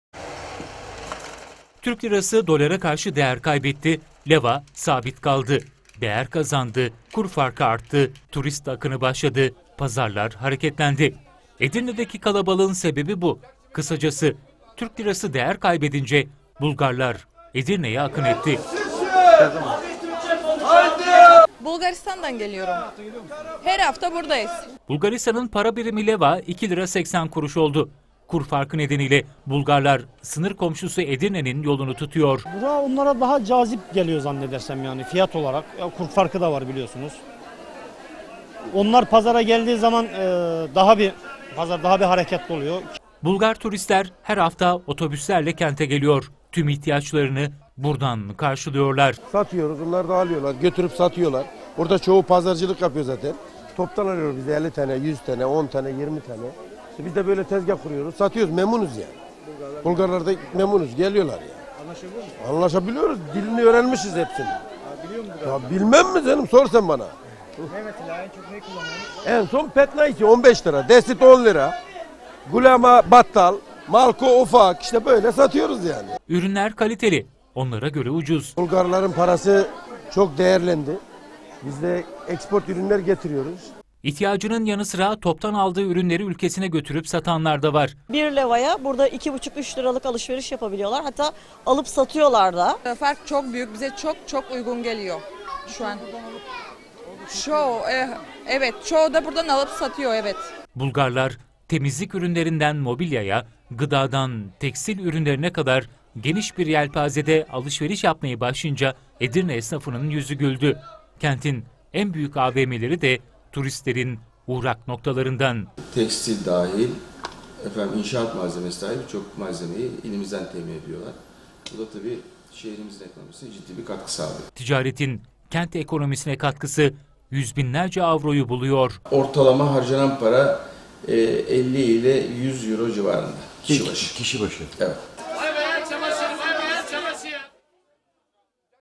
Edirne'ye Bulgar akını
Türk parasının dolar karşısında değer kaybetmesiyle Bulgaristan para birimi Leva, TL'ye karşı değer kazandı. Hal böyle olunca sınır kenti Edirne'ye Bulgar turist akını oldu. Kur farkı nedeniyle daha ucuz alışveriş fırsatı yakalayanlar haftasonları Edirne'yi tercih ediyor.